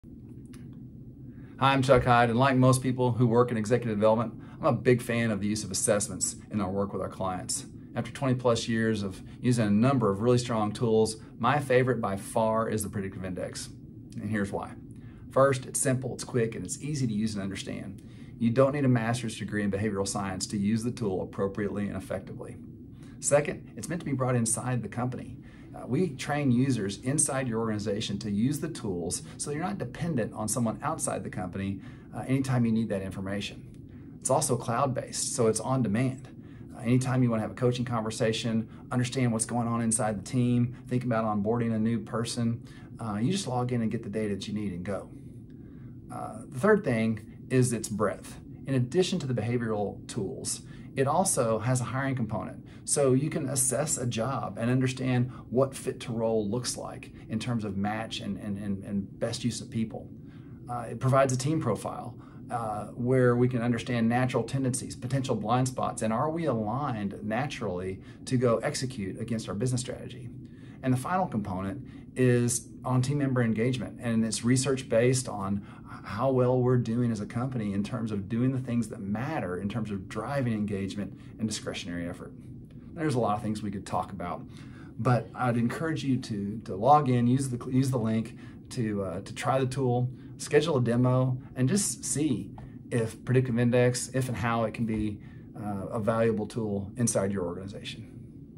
Why PI
After more than 20 years of using all types of assessment instruments, the Predictive Index is my favorite by far.  Here, I give three practical reasons on why I've chose it to be a core part of my coaching and consulting practice.